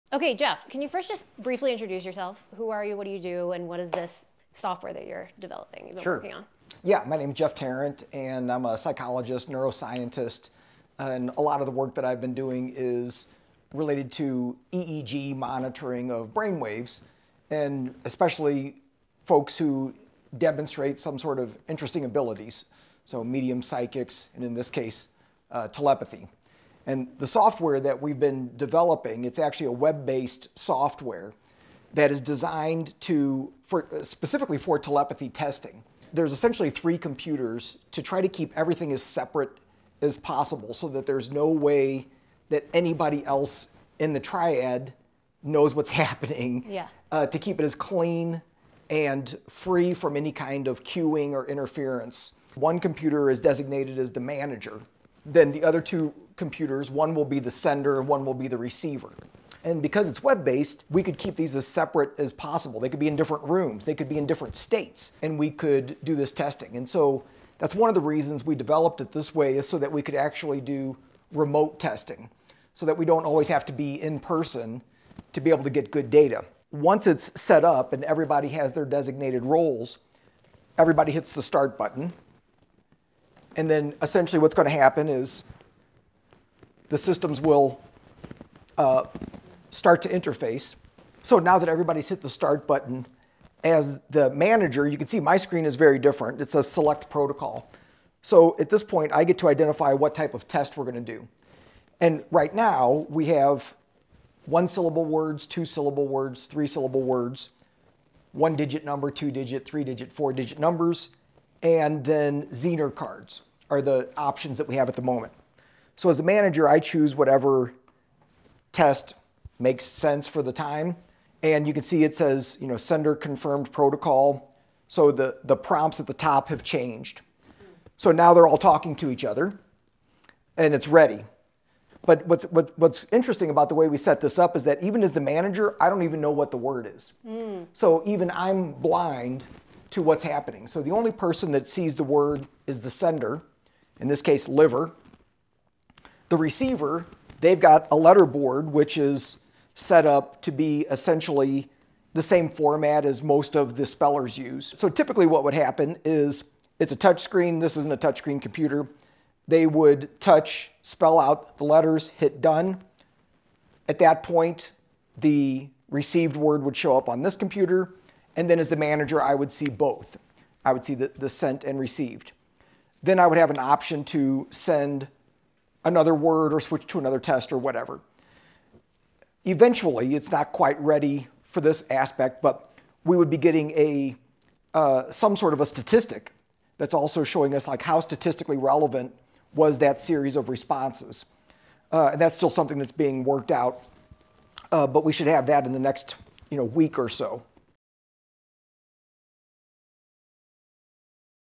Jeff Tarrant's Double Blind Software
In this video, Dr. Jeff Tarrant walks us through the groundbreaking software called ESP Metrics he’s developed with David Joffe to double-blind test telepathic communication—technology now being used in ongoing research with nonverbal individuals like Trevor.

This demonstration is part of Talk Tracks Episode 15: “Testing Telepathy: Trevor’s Story and a New Scientific Tool.” The episode explores the intersection of neuroscience, software innovation, and extraordinary communication between a mother and her nonverbal autistic son.